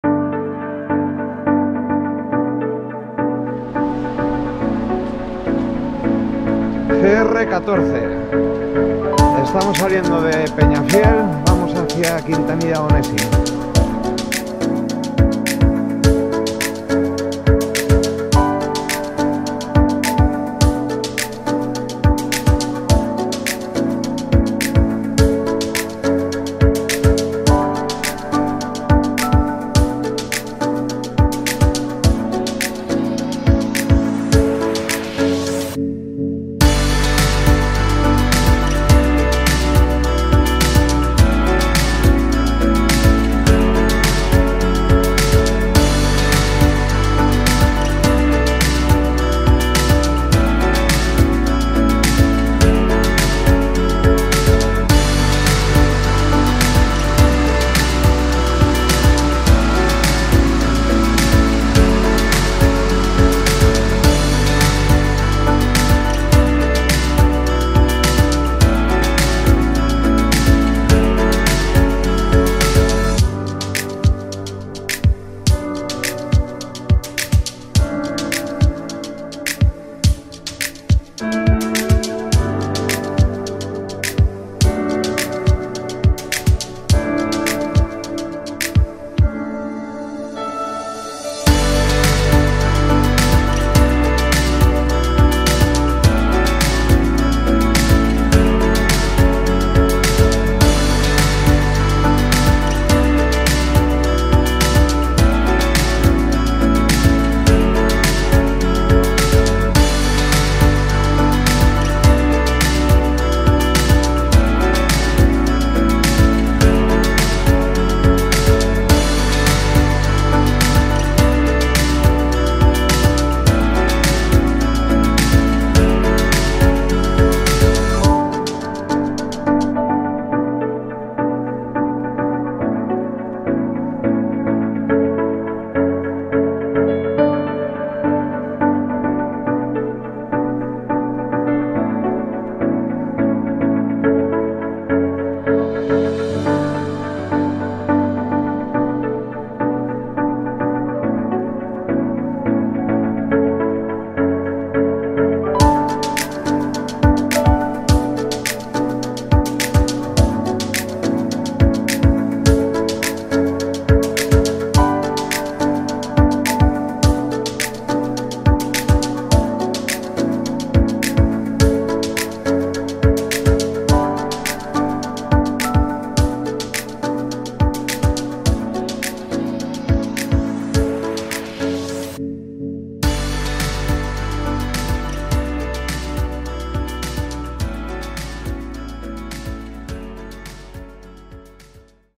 La esencia de la SENDA del Duero | PEÑAFIEL
La esencia de la Senda del Duero
El Camino Natural enlaza varias poblaciones ligadas al río integrando los valores ambientales de la ribera con su patrimonio histórico-cultural y vitivinícola. La senda sigue al Río Duero y a uno de sus afluentes, el Río Duratón, a lo largo de treinta y un kilómetros. Su recorrido aprovecha las antiguas sendas de los pescadores, y recupera de esta forma los senderos tradicionales que recuerdan al viajero las actividades en las que el río era el protagonista: La pesca, los pasos de barca, los molinos, las aceñas y los puentes.